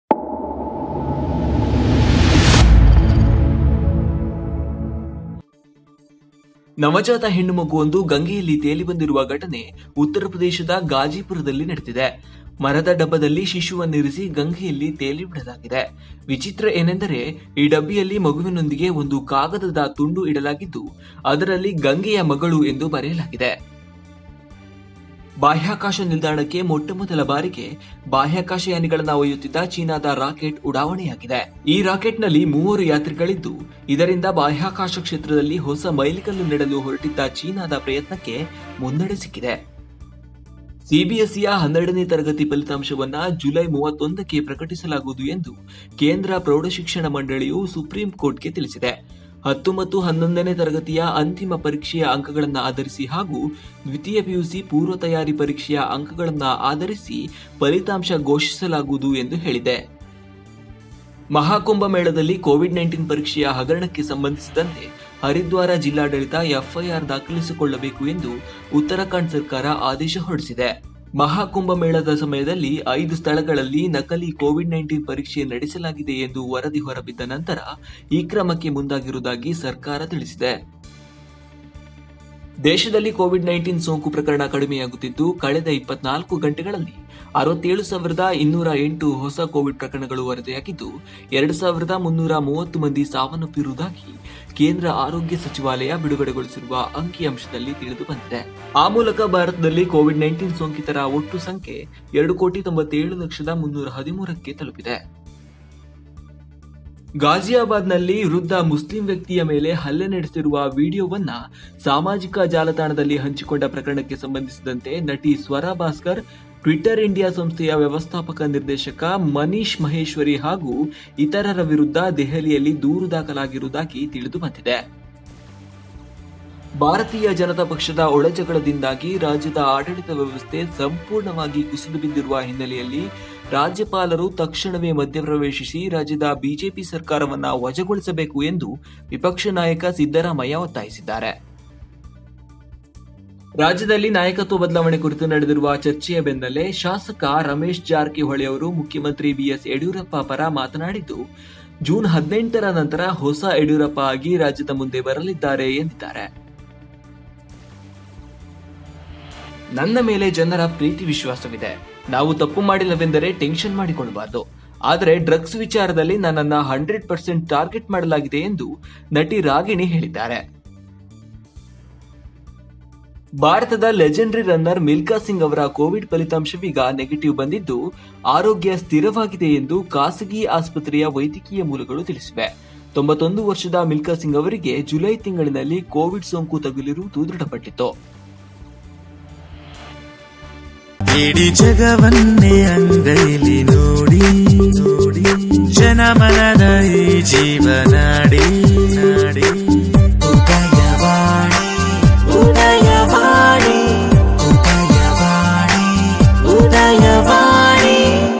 ಆರೋಗ್ಯದಲ್ಲಿ ಸ್ಥಿರ : ಐಸಿಯುನಿಂದ ಹೊರಬಂದ ಮಿಲ್ಕಾ ಸಿಂಗ್ | Udayavani News Bulletin 17-JUN-2021
For more..